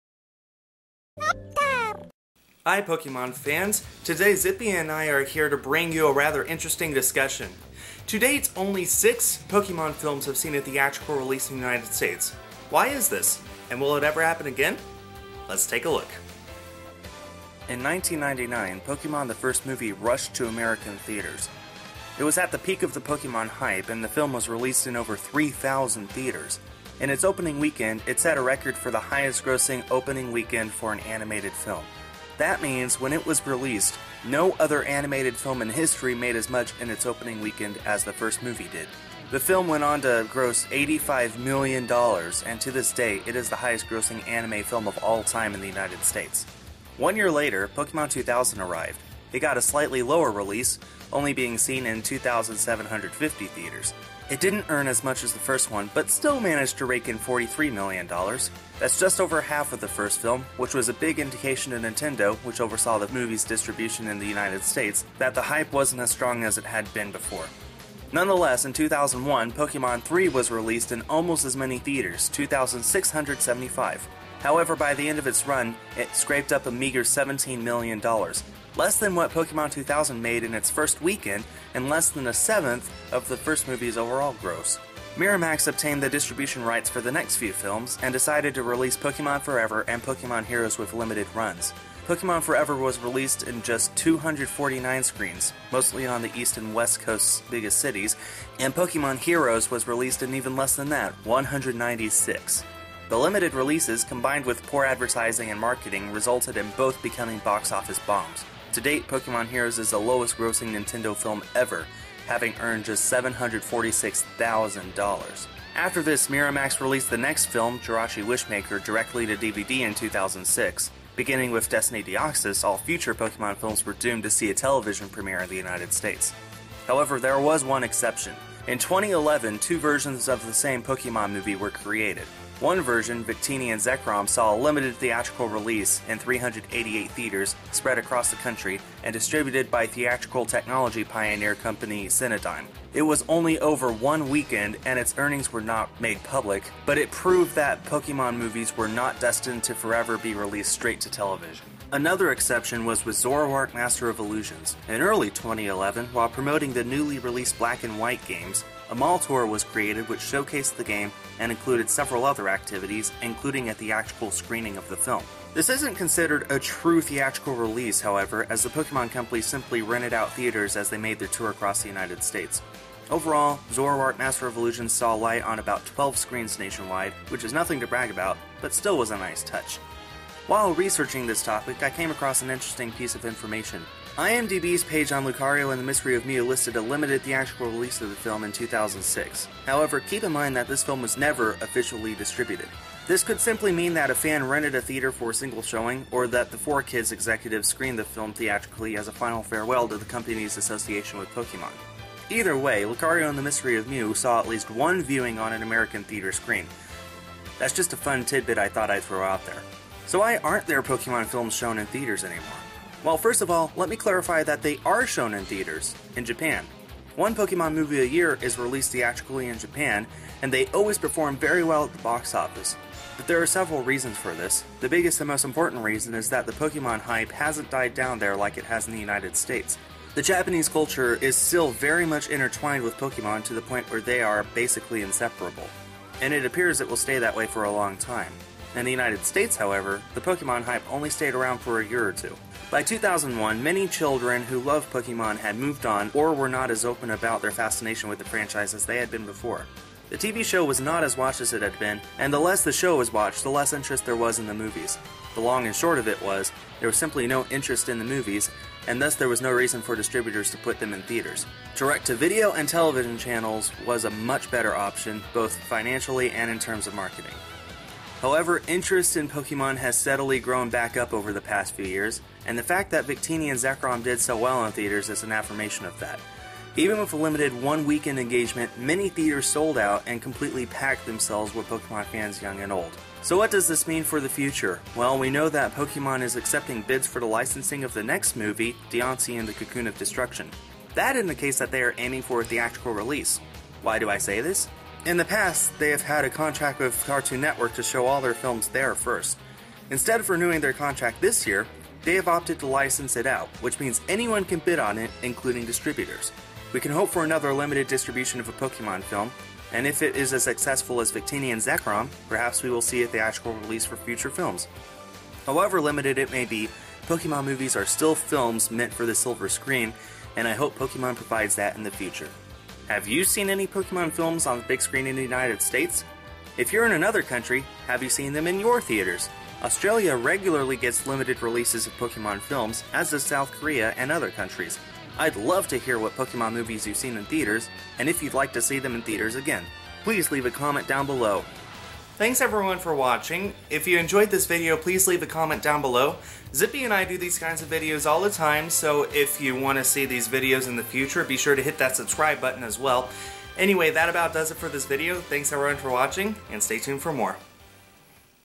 Pokemon Discussion of the Day - Theatrical Films
Why aren't Pokemon films shown in theaters anymore? And will that change? Today, that's exactly what Zippy and I investigate!